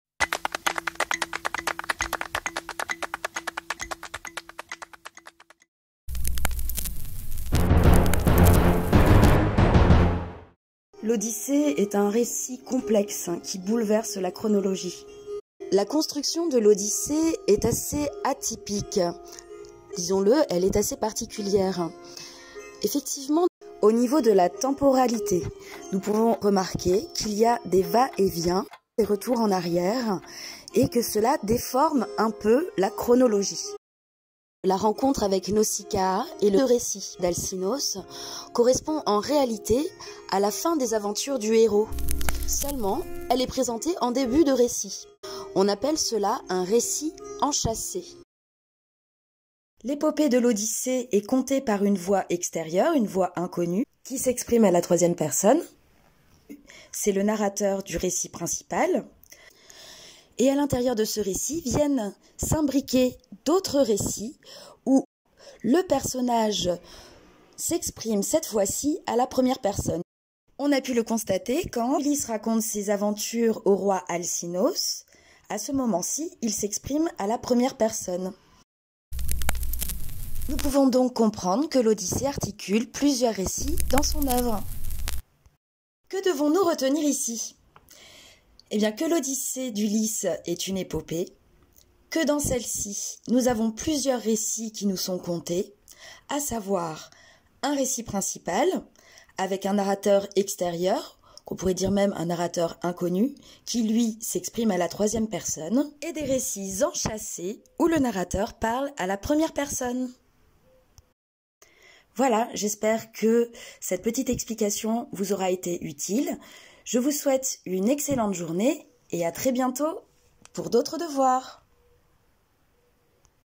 La construction du récit de l'Odyssée - Une épopée sous forme de récit enchâssé
Le récit enchâssé dans le récit de l'Odyssée d'Homère

Transcription :
Structures et quelques mots clefs
L'Odyssée est un récit complexe qui bouleverse la chronologie. La construction de l'Odyssée est assez atypique. Disons-le, elle est assez particulière.
Effectivement, au niveau de la temporalité, nous pouvons remarquer qu'il y a des va-et-vient, des retours en arrière et que cela déforme un peu la chronologie.
La rencontre avec Nausicaa et le récit d'Alcinoos correspondent en réalité à la fin des aventures du héros. Seulement, ces aventures sont présentées en début de récit.
On appelle ce procédé d'écriture : RECIT ENCHASSE

L'épopée de l'Odyssée est comptée par une voix extérieure, une voix inconnue, qui s'exprime à la troisième (3ème) personne. C'est le narrateur du récit principal.
Et à l'intérieur de ce récit, viennent s'imbriquer d'autres récits où le personnage s'exprime cette fois-ci, à la première (1ère) personne.
On a pu le constater quand Ulysse raconte ses aventures au Roi Alcinoos, à ce moment-ci, il s'exprime à la première (1ère) personne.

Nous pouvons donc comprendre que l'Odyssée articule plusieurs récit dans son oeuvre.

Que devons-nous retenir ici ?
1) L'Odysée d'Ulysse est une épopée
2) Il y a plusieurs récit comptés dans celle-ci, à savoir un récit principal (avec un narrateur extérieur, inconnu qui s'exprime à la 3ème personne) et des récits enchâssés (où le narrateur parle à la 1ère personne du singulier)

Support pédagogique réalisé pour une classe de 6ème, dans le cadre du télé-enseignement, suite au confinement dû à la Covid19.

Montage/ réalisation par mes soins.